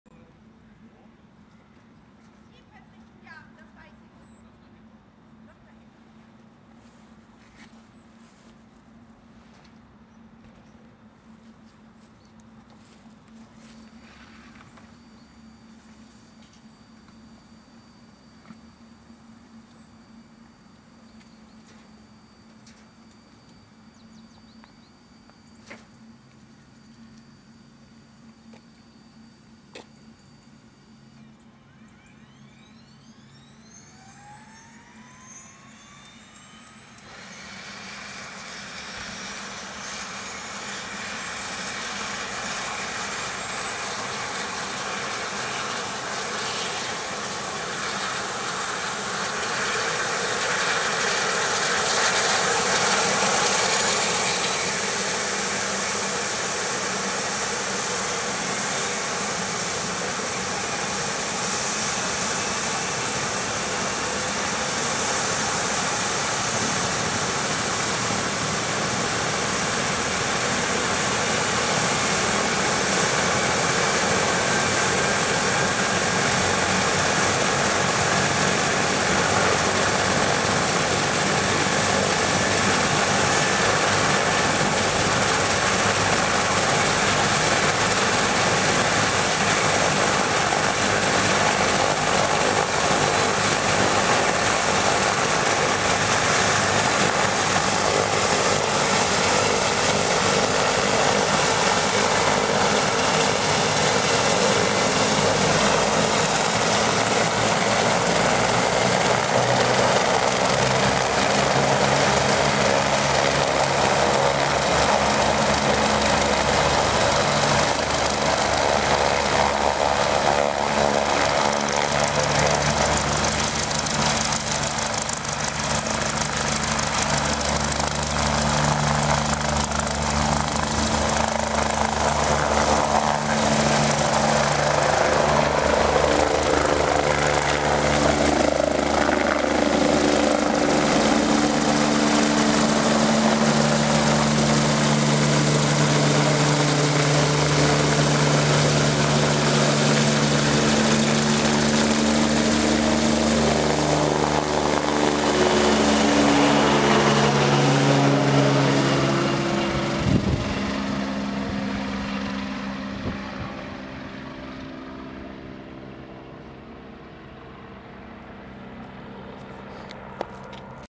Christoph 47 Startet Richt. Demmin/Jarmen
am 18,10.09 gegen 12 Uhr
Mal wieder nen Spottertag am LRZ verbracht , habe dort auch meinen Partner Toni getroffen .
Bitte bewerten bzw, kommentieren :)